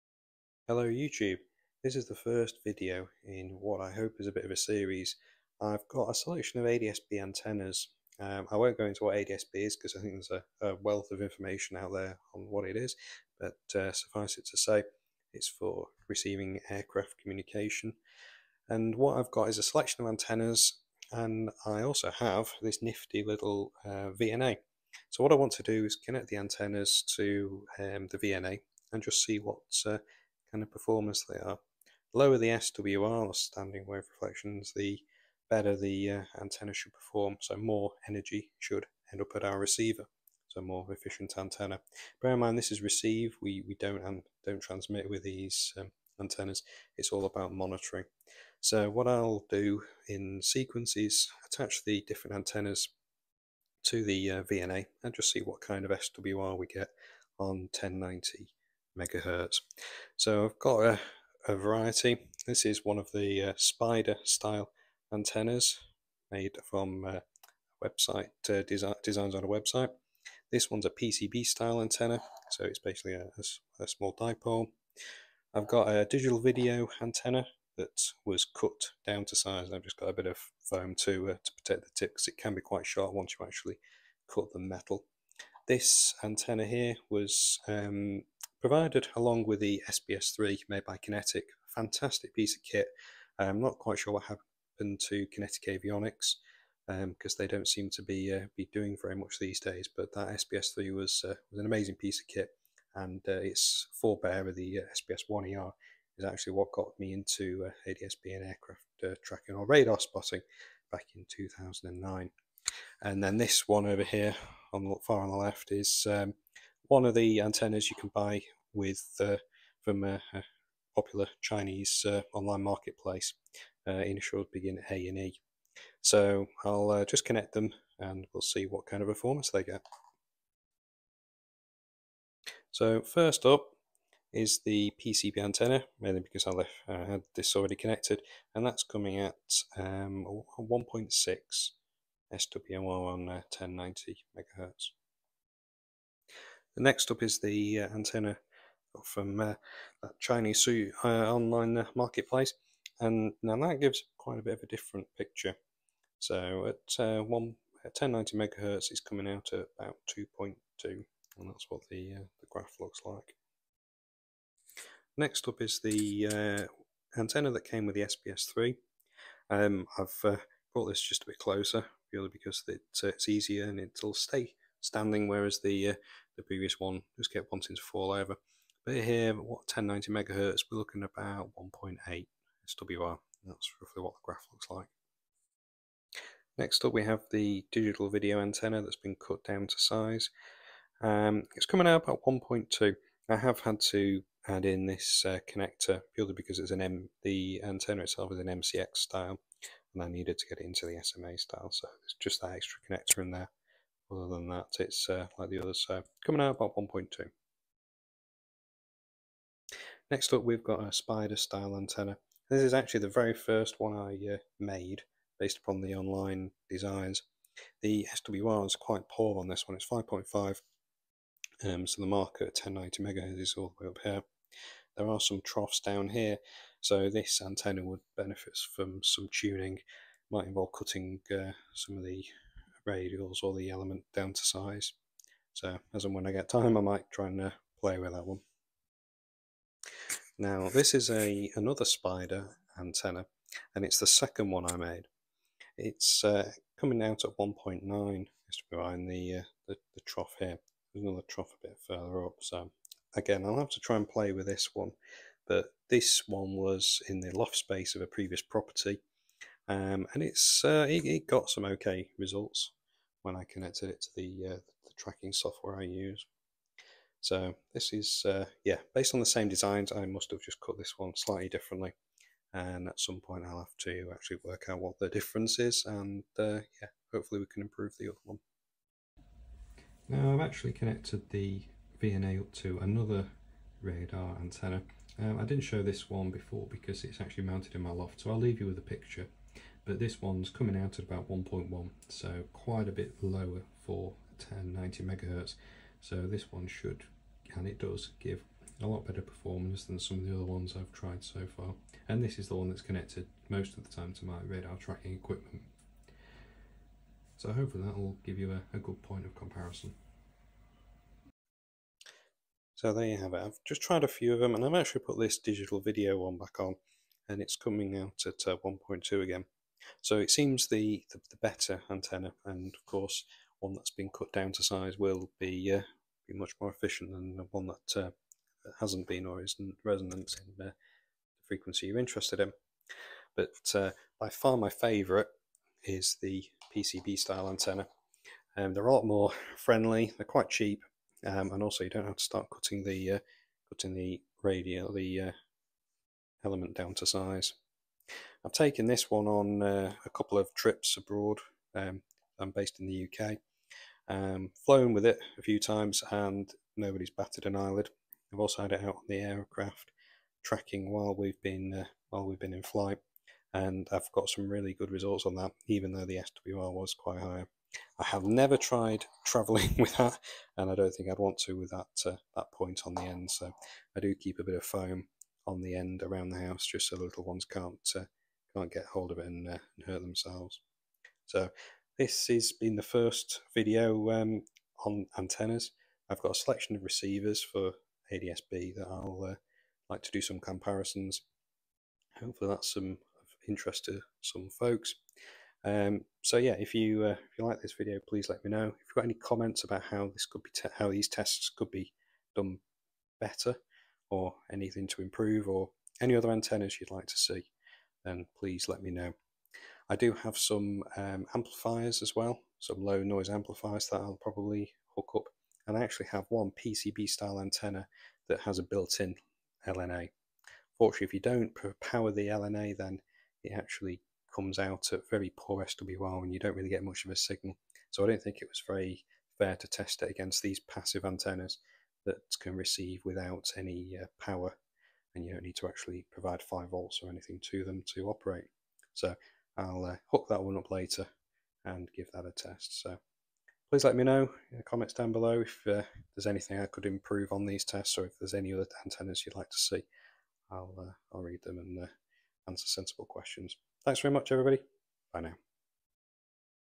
ADS-B Antennas
Hi everyone!
In this video I take a look at a variety of ADS-B antennas hooked up to a Vector Network Analyser (VNA), to see what kind of SWR they have. SWR, or Standing Wave Ratio (correction to the video), is a measure of the efficiency of the antenna, the lower the SWR, or closer to 1:1 you can get, the better the antenna is on the frequency you're interested in, which for us here is 1090MHz.

The commercially made antenna mounted in the loft is a RADAR-200 colinear style antenna bought from Moonraker (UK) Ltd back in 2016.

If there are any antennas that you'd like to see tested, please let me know down in the comments.

Coming soon, a comparison of ADS-B receivers/dongles such as the FlightAware PRO stick, and a standard DVB-T receiver, and a look into some Low Noise Amplifiers (LNAs) for 1090MHz.

If you've got any suggestions on how the tests could be improved, please let me know down in the comments.

Update 18/09/2024: I've been asked about some of the equipment in the video.
- The AliExpress antenna with the coils is currently available
- The SBS-3 looks to have been discontinued (from what I can tell, anyway), however Radioworld UK still has a second hand link (currently showing out of stock) which would give you an idea of what the unit looks like
- The digital video antenna cable came as a bundle with a DVB-T receiver, and that's something I'll be showing in my upcoming video on receivers
- The spider antenna can be made using the link to flightaware above.
- I can't find the RADAR-200 colinear antenna I have in my loft space, however this is a similar one from Radioworld